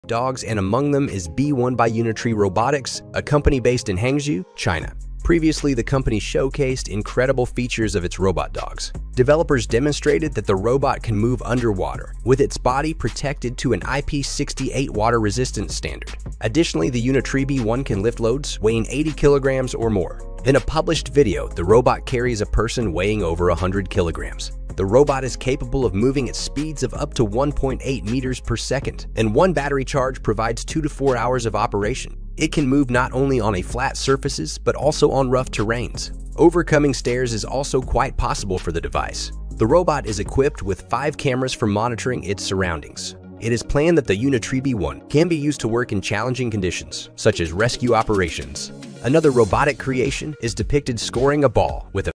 China's Robotic dogs for challenging conditions
The World Robot Conference 2023 has kicked off in China. This is truly one of the grandest events of this year in the field of robotics, taking place in Beijing. The event is being held at the Etrong International Exhibition and Convention Center. More than 140 Chinese and foreign companies brought about 600 exhibits to the World Robot Expo.